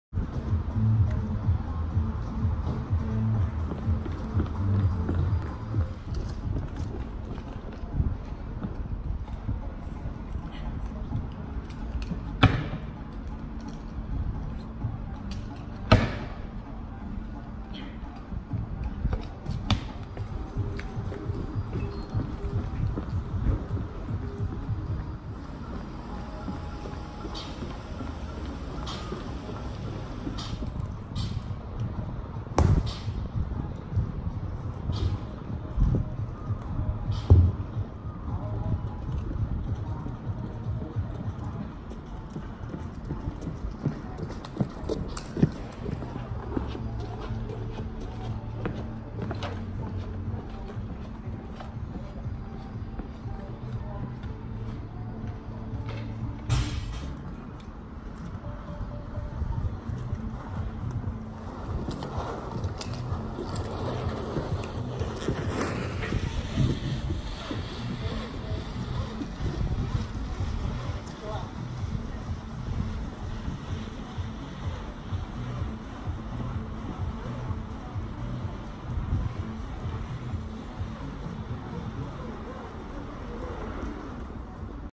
British Firefighter Challenge Training
I’m participating in the British Firefighters Challenge next Sat 27th July 2019, so thought I’d give some similar training a go.

We don't have any stairs I could go up, so I ran across the yard/carpark instead, and did ten slam balls to replicate hauling aloft. I also didn't wanna drag the couplings along the floor, so ran out the hose like normal (but I'm shit at that anyway, so need the practise!) Rolling up hose with a set on is pretty hard! I missed out the container carry. Oops (my own fault for not checking the challenges before I started). The dummy drag felt ok, not as heavy as the 70kg has felt before.

It's all for charity, and I'm convinced I'm going to come last (well somebody has to), but I'm just going to try and have fun (in those few minutes of hell. lol)